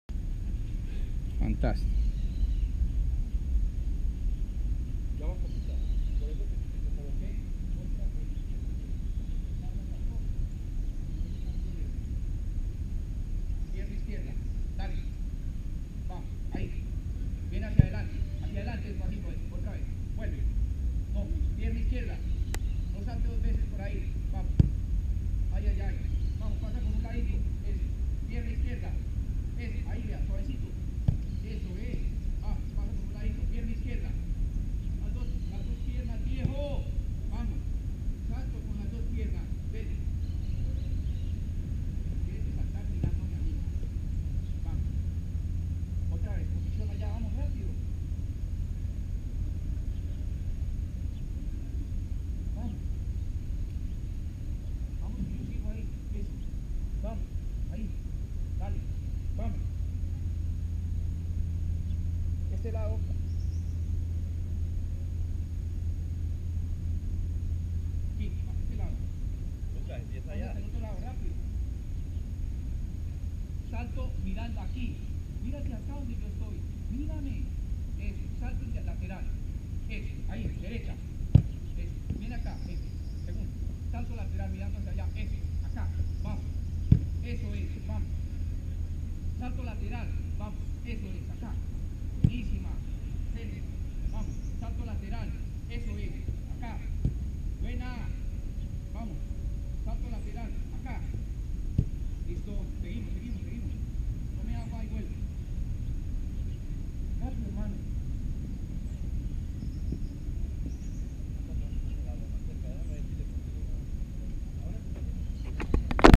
Martin viveros desplazamiento lateral salto valla y golpeo # 5 Lucas Norte-16 de septiembre -2017
Martin viveros desplazamiento lateral salto valla y golpeo # 5 con Lucas Norte-16 de septiembre -2017